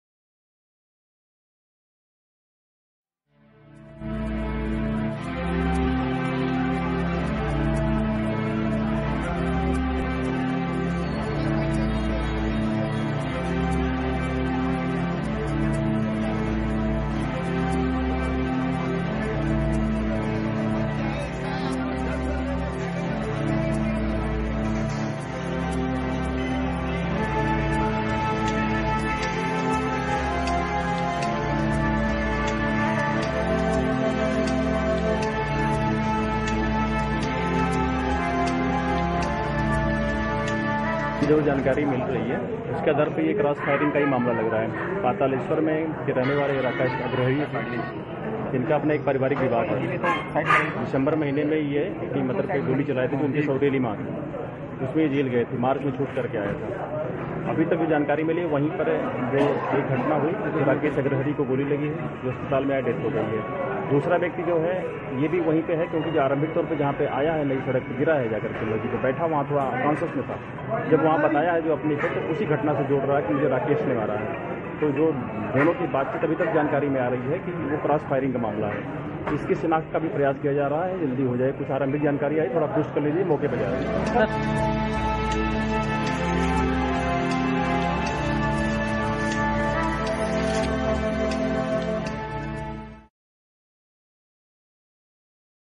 Two person shot dead in cross firing in Varanasi
Two person died in cross-firing in Varnasi's Devnathpura. An alleged Kanpur based history-sheeter fired at businessman Rakesh Agrahari on not receiving a positive response for his demand for extortion.